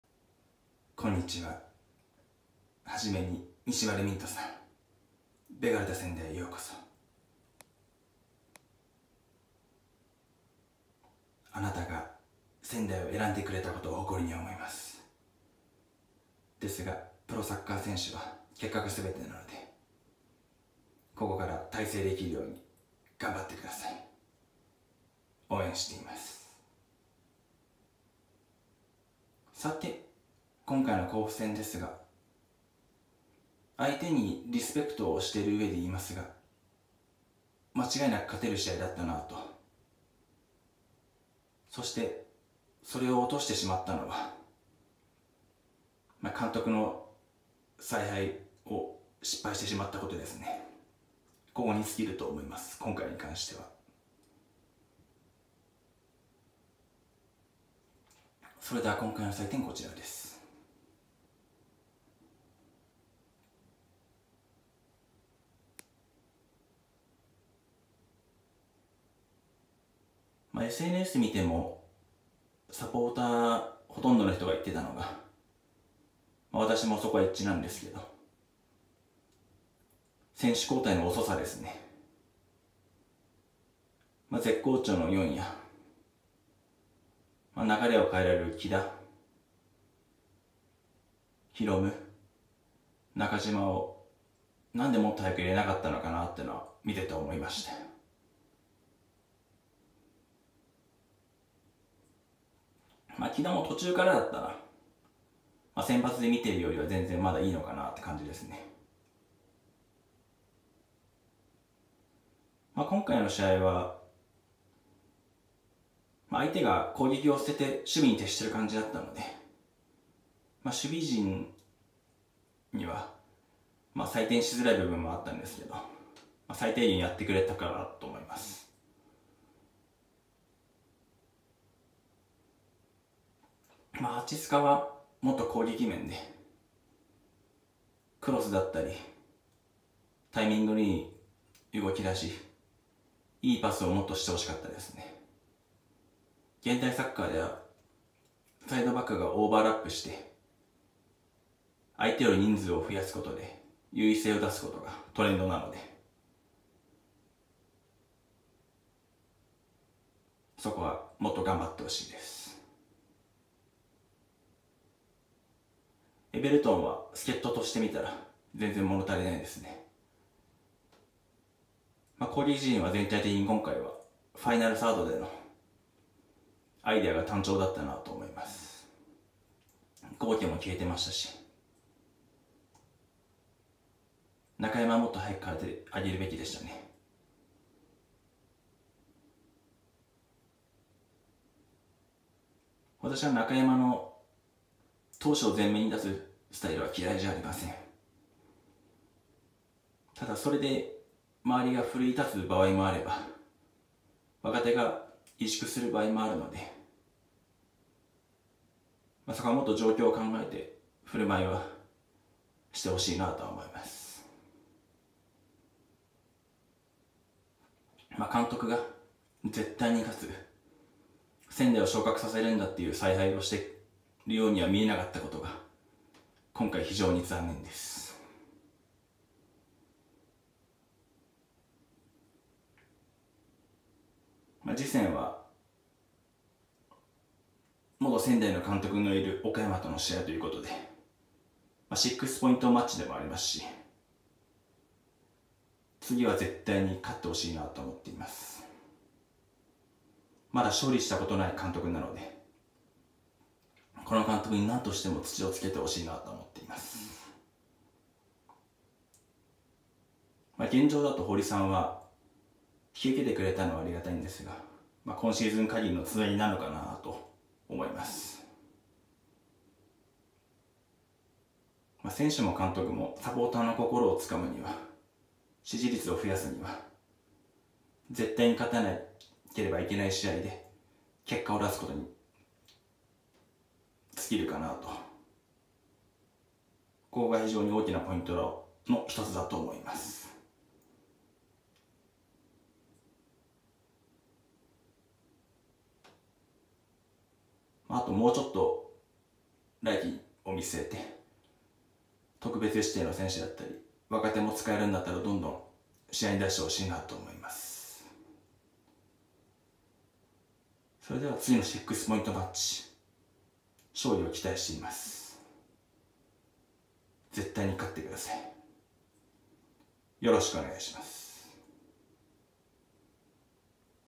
【勝利厳命】ベガルタ仙台 第33節振り返り&第34節展望 vs ファジアーノ岡山FC
0.5刻みの10点満点で「6」が及第点です。
※出場時間が短い選手は書いていません。

▽MONKEY MAJIKさんの動画はこちら
公式待ち